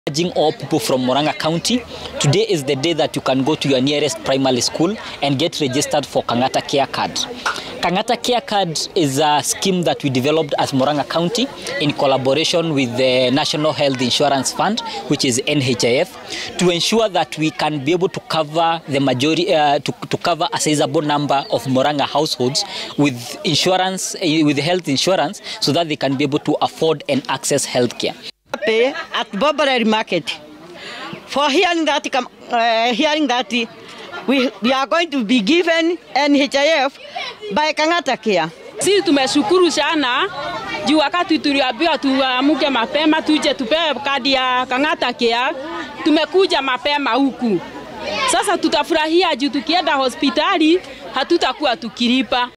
MRADI WA KANG'ATA WA AFYA
Kaunti ya Murang'a yatenga 80M ili kuimarisha sekta ya afya.